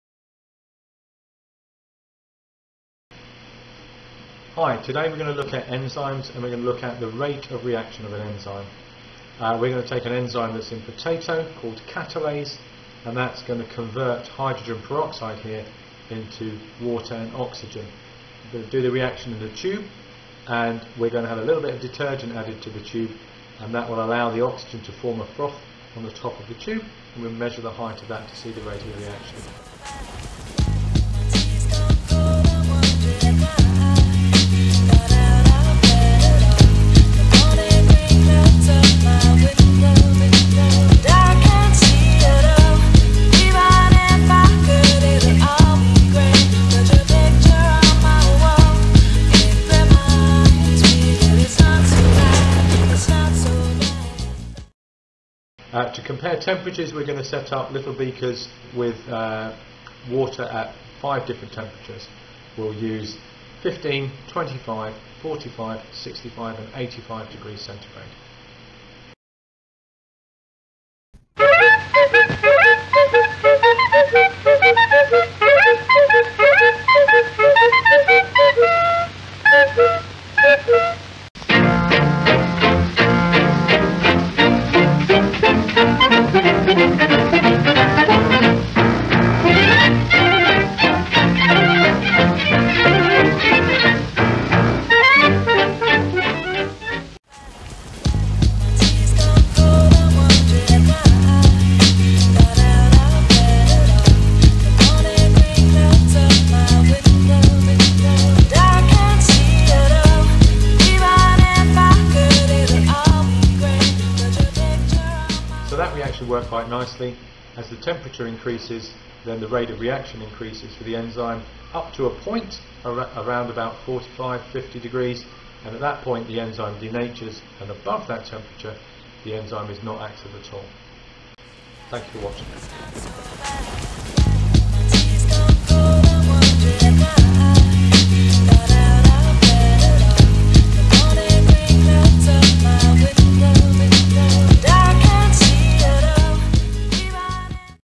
How temperature affects enzyme rates (version 2)
Using catalase and hydrogen peroxide to investigate the rate of enzyme reaction at different temperature. A short video showing the basic idea